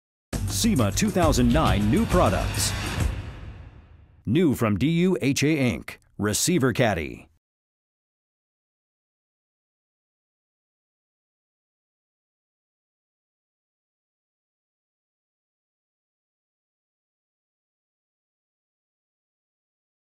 Receiver Caddy from DU-HA Inc. ID6674
SEMA SHOW 2009 NEW VAN/PICKUP/SPORT-UTILITY PRODUCT:  The Receiver Caddy is designed to hold ball mounts securely in the bed of pickup trucks and the rear cargo area of SUVs.